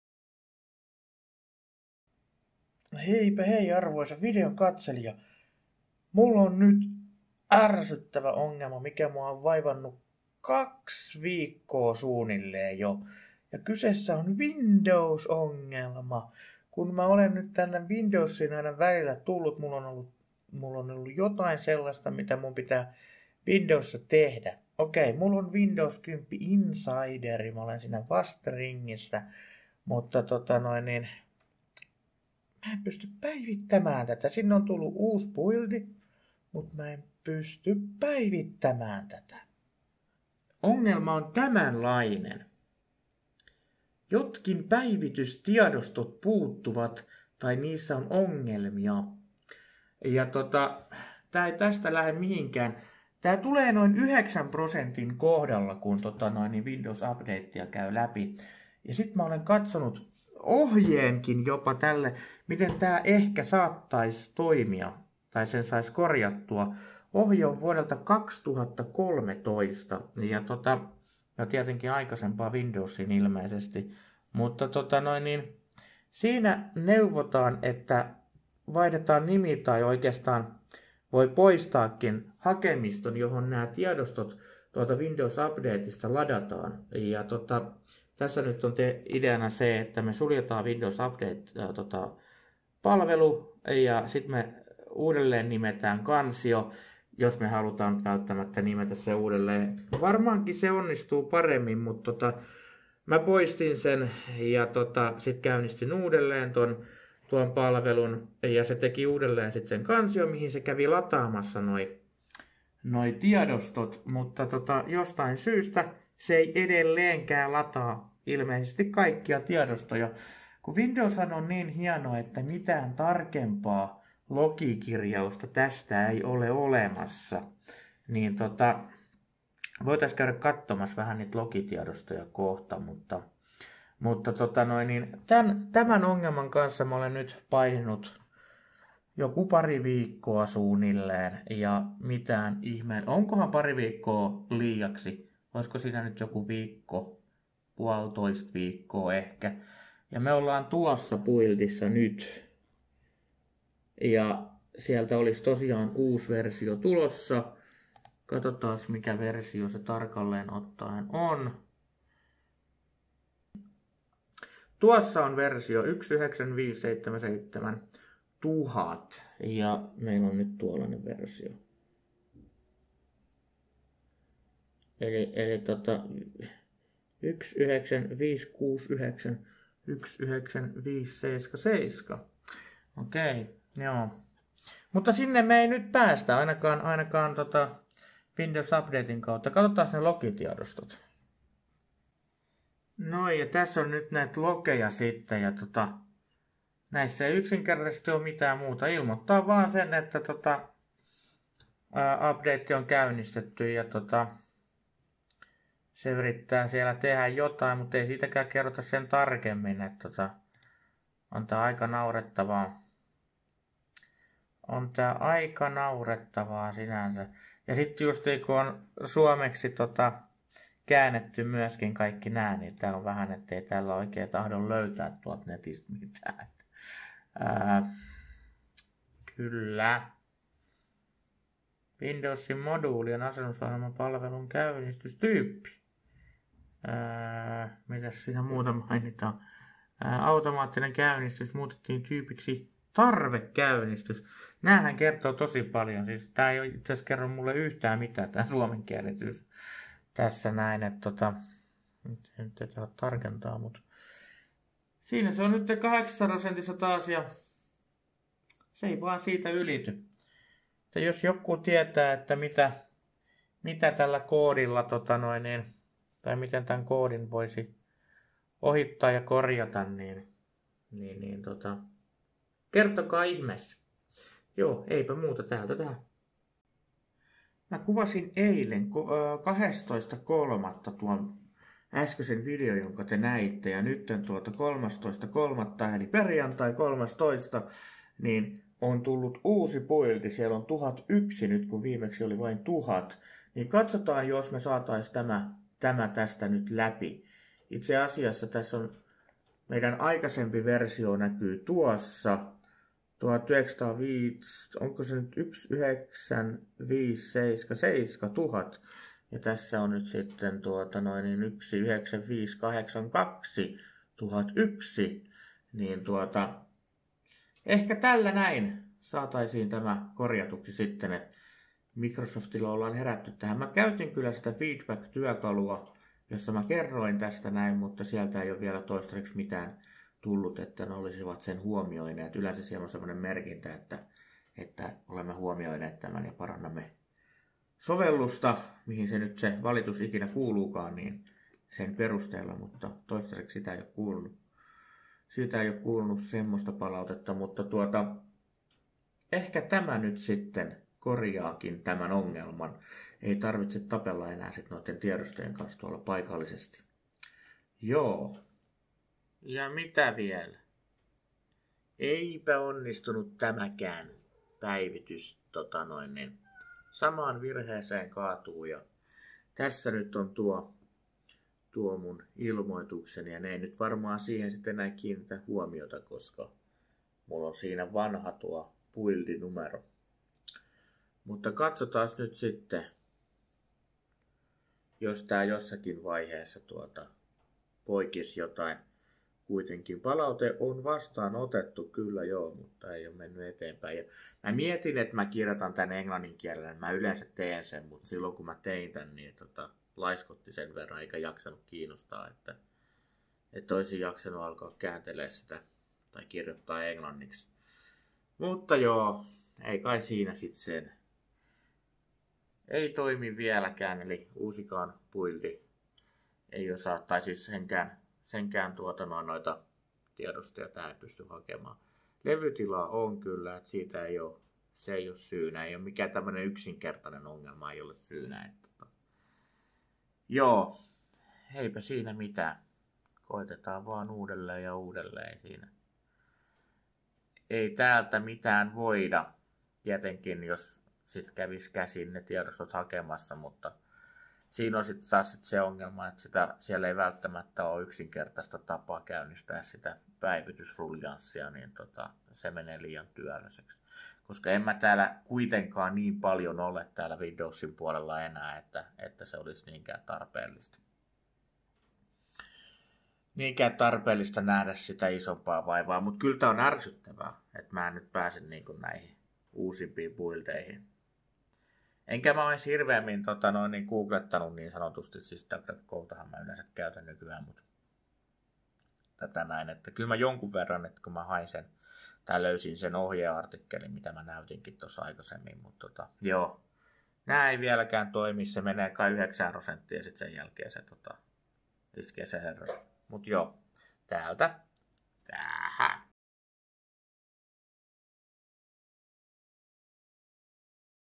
Windowsin virhekoodi 0x80070003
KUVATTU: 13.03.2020
On totta, että Windowsia on kympin jälkeen haukuttu ja parjattu siitä, että se pakottaa päivitykset käyttäjien tietokoneille ja vielä menee käynnistämään tietokoneen automaattisesti uudelleen.

Noh, en usko, että Microsoftilla olisi tähän palautteeseen juurikaan reagoitu mutta minun tapauksessani, Windows ei todellakaan hae ja asenna päivitystä 😡.

Windows update törmää jatkuvasti virhekoodiin: 0x80070003. Suomenkielisessä Windowsissa ei virhelokeista juuri hyötyä ole, eikä Windows updaten selityksestä, jossa mainitaan seuraavasti: ”Jotkin päivitystiedostot puuttuvat, tai niissä on ongelmia. Päivityksen lataamista yritetään myöhemmin uudelleen. Virhekoodi: (0x80070003)”

Mikäli joku tämän videopostauksen nähnyt, tietää kuinka tämä ongelma korjata, voi ystävällisesti vinkin korjauksesta heittää kommenttikenttään.